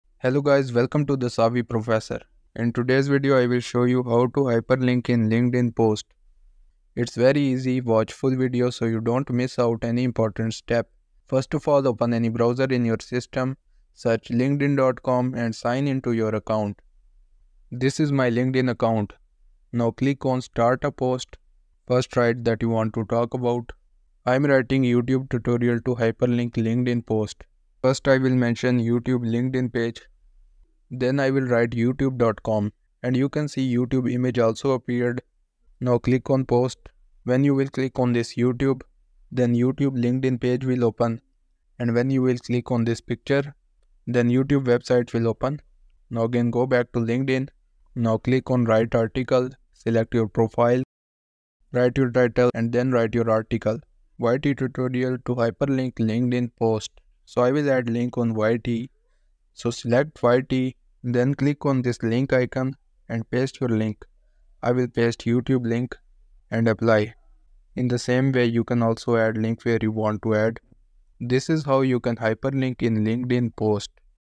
How to Hyperlink in LinkedIn Post (Best Way to Add Hyperlink in LinkedIn Post)
How to Hyperlink in LinkedIn Post (Best Way to Add Hyperlink in LinkedIn Post). In this tutorial, you will learn how to bookmark a website on Microsoft Edge.